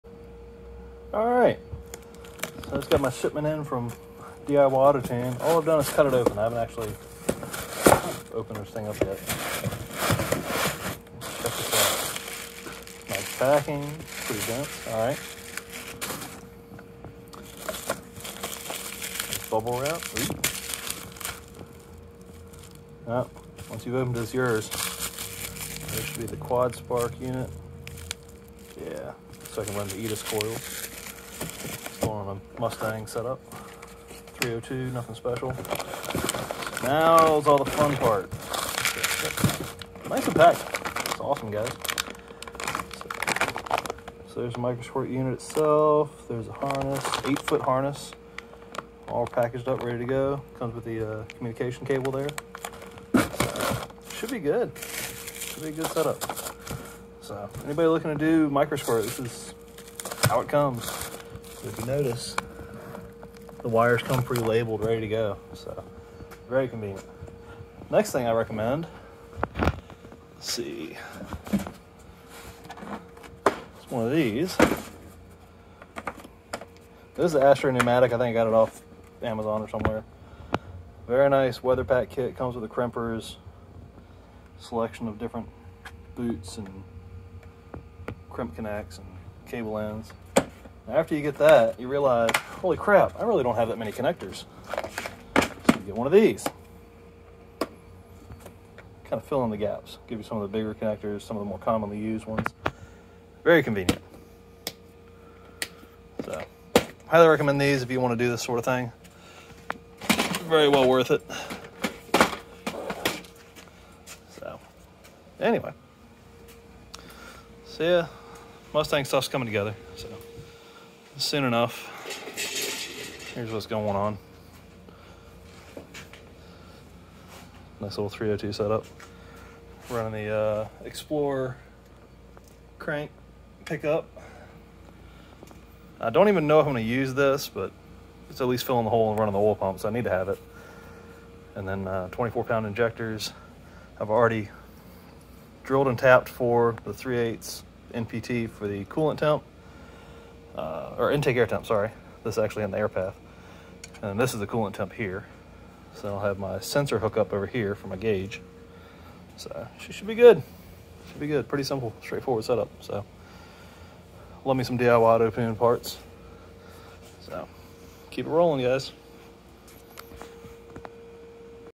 DIYAutoTune Microsquirt, and QuadSpark unboxing!
I love DIYAutoTune products! This will be running a basic 302 setup in my 86 mustang LX, with the 96 explorer crank pick up and the 4 pack EDIS coils!